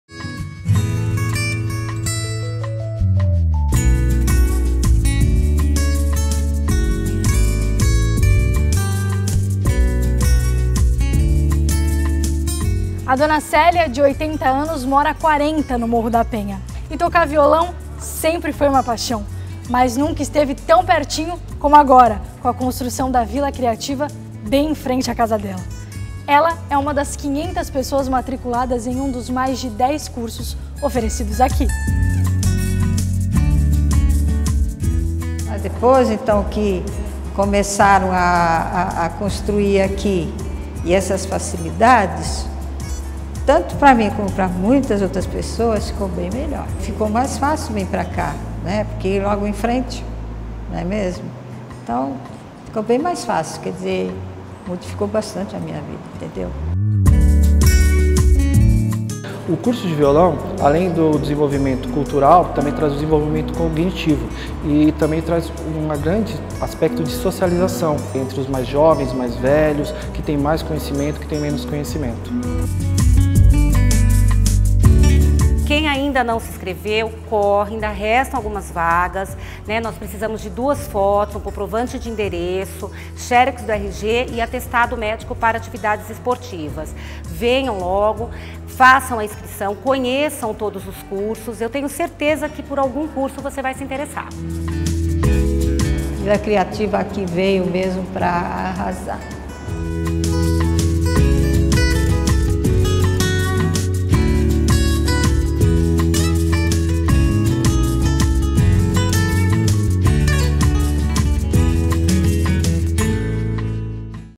Aulas de violão mudam a vida de Dona Célia
Conheça a história da aposentada que é uma das alunas do curso de violão da Vila Criativa do Morro da Penha. As inscrições para este e outros 13 cursos estão abertas. Saiba como se matricular acessando o nosso portal Reportagem de Isabella Pajón.